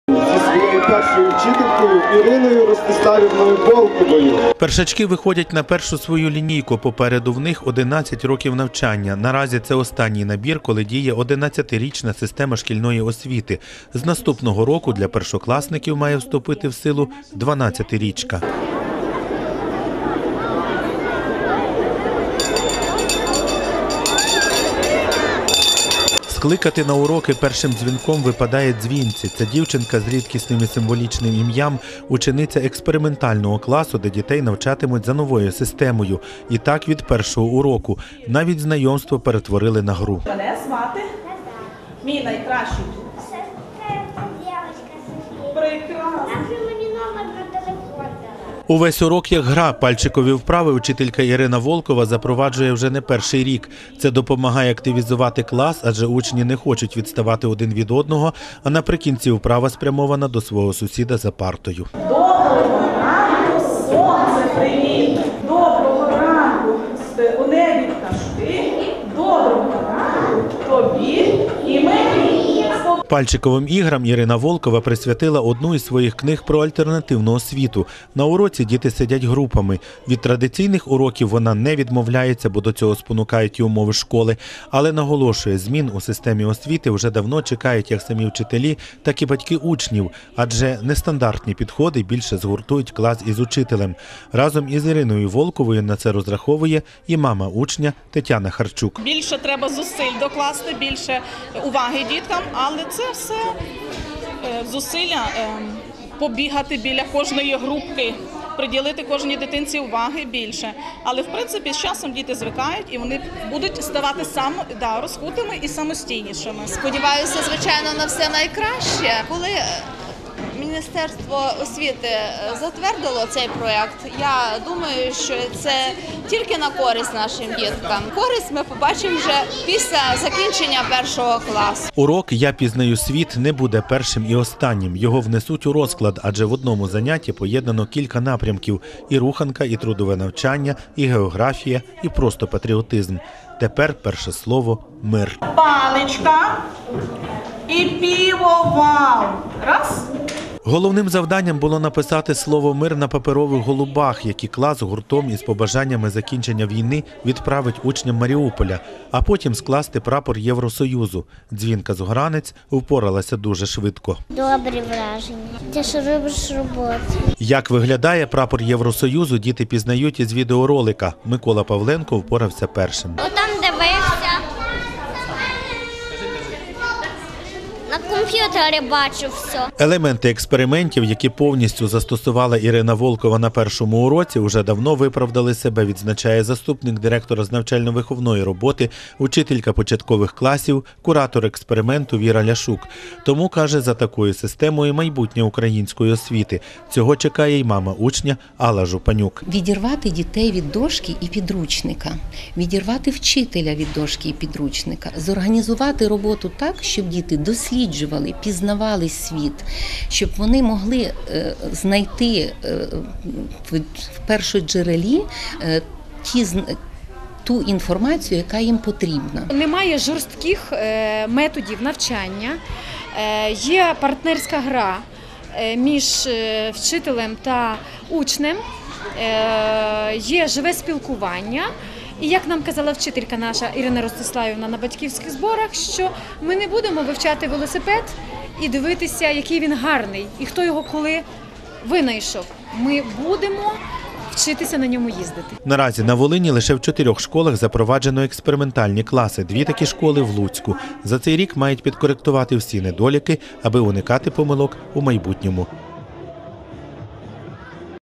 1 вересня в експериментальному класі. На Волині такі є в чотирьох школах
Перший раз у перший клас – і одразу експериментальний. Від сьогодні частина першачків навчатимуться за іншою системою. Певні уроки будуть інтегровані, діти почуватимуться вільно, а вчитель має бути старшим другом для дитини і її поводирем у невідомий світ. Репортаж із першої луцької школи.